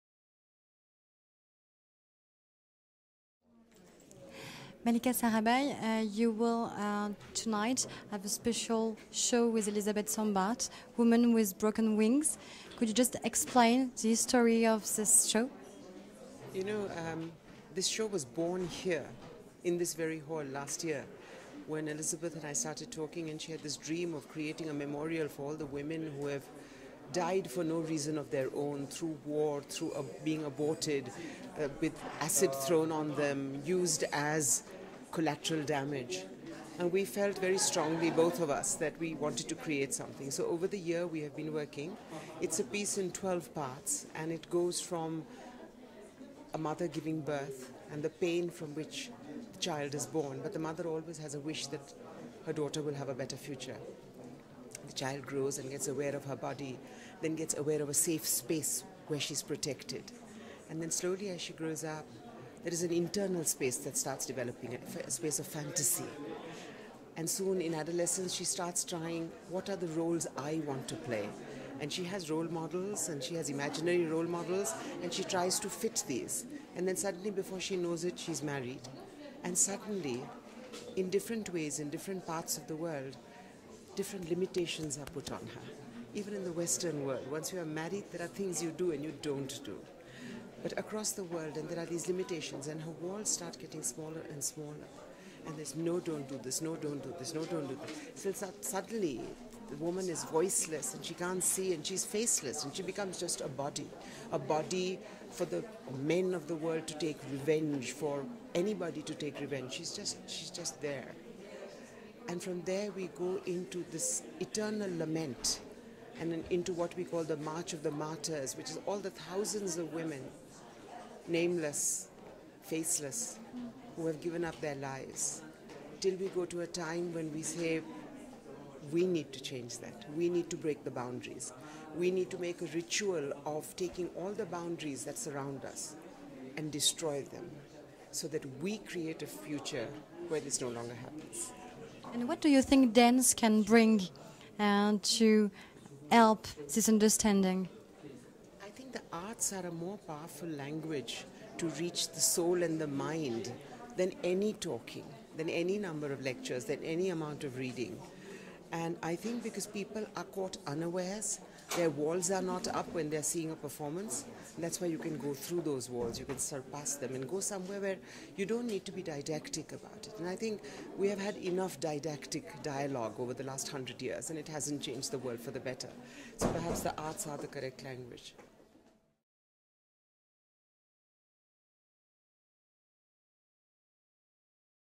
Malika Sarabhai, Darpana - Exclusive Interview at Zermatt Summit 2012
Artivist Educated as an economist and a business manager, Mallika Sarabhai is one of India's best-known classical dancers. She is Director, Darpana Academy of Performing Arts, a centre for the arts where the focus is on the use of arts for social change.

As dancer, actress, choreographer, writer, social entrepreneur and instigator of community projects, Mallika Sarabhai has fearlessly challenged audiences to sit up and think, and to realign themselves to critical questions of gender bias, communal hatred, the environment, corruption, violence and more.